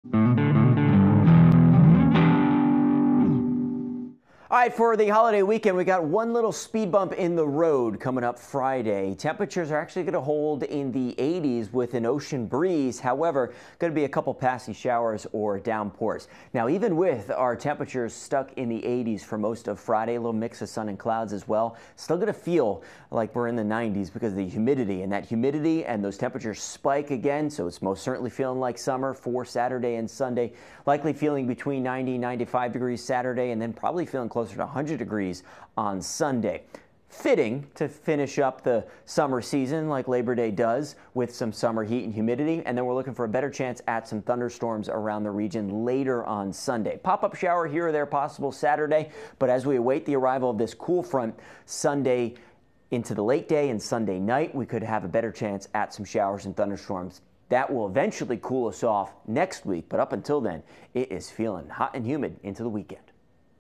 Steve's Setlist: Humid holiday weekend
Humid for most of the holiday weekend with a few chances of rain.